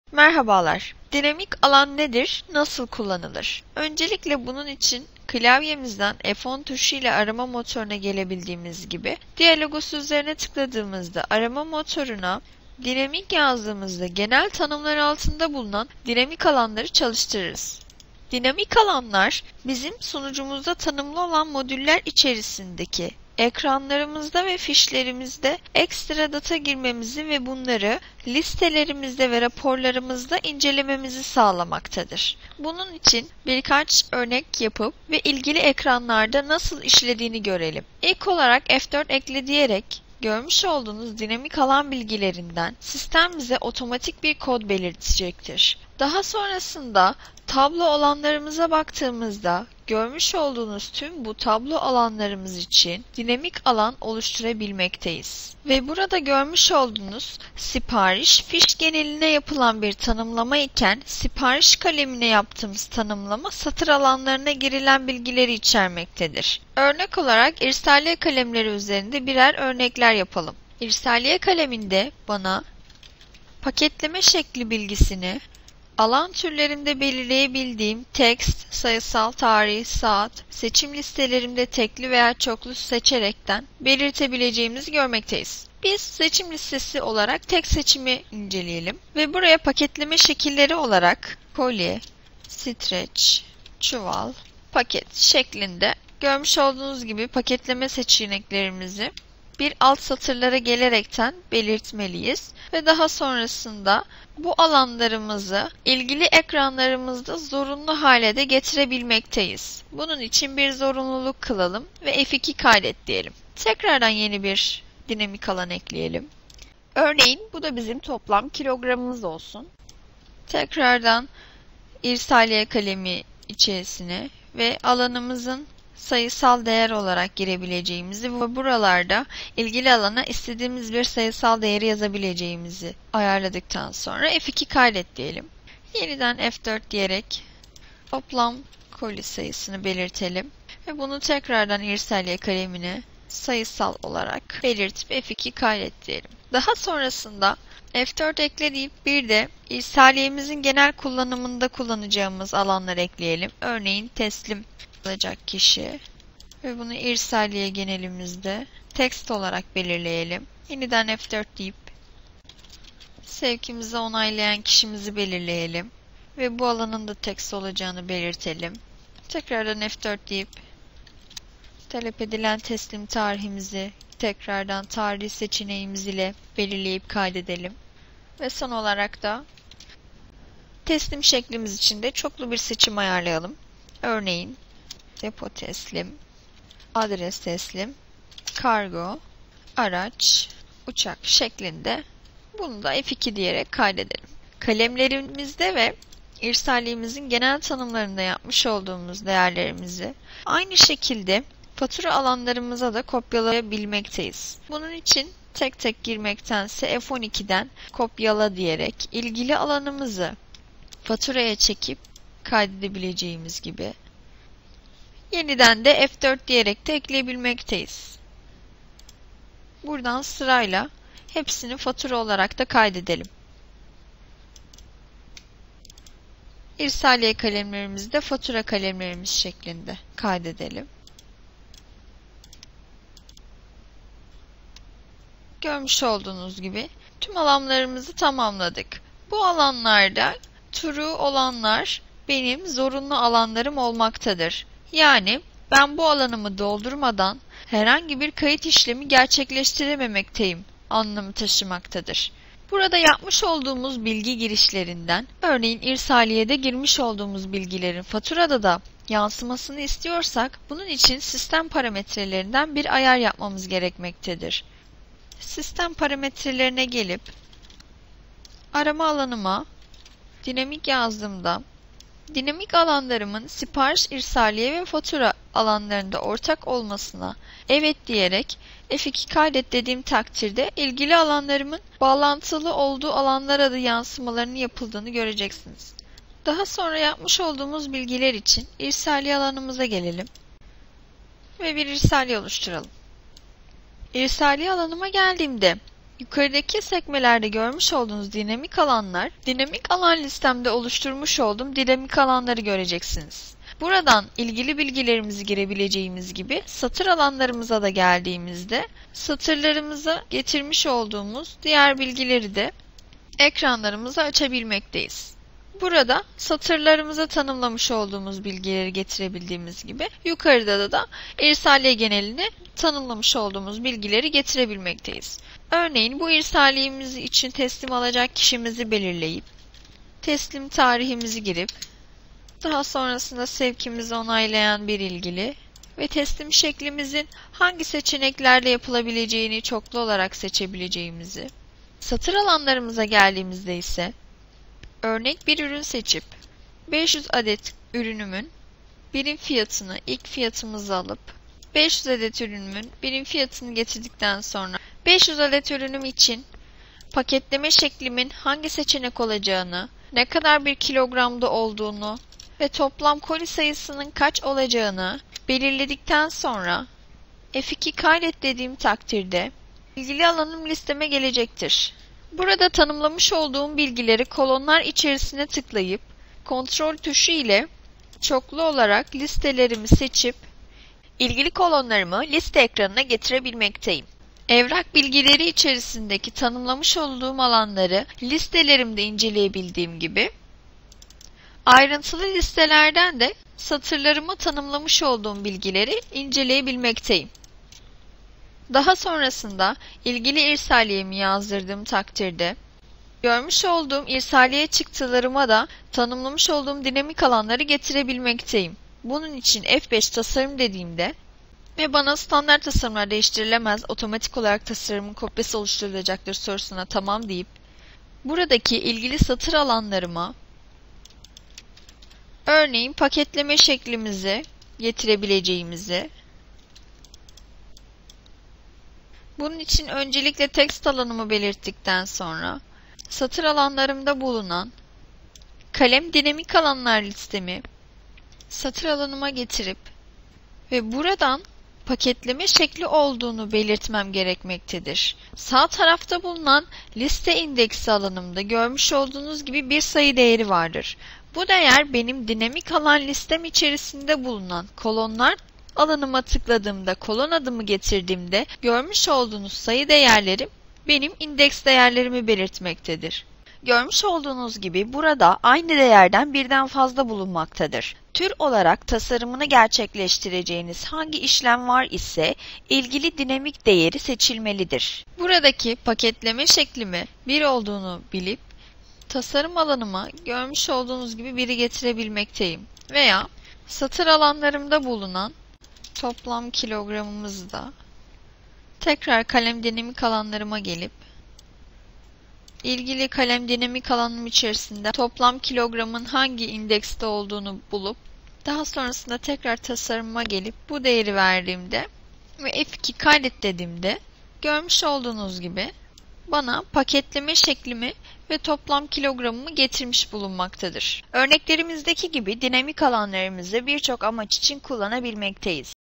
DİA Akademi - Dinamik Alanlar Nasıl Tanımlanır ve Kullanılır?
DİA Akademi - Dinamik alanların nasıl tanımlandığını ve nasıl kullanıldığını anlatan eğitim videosudur.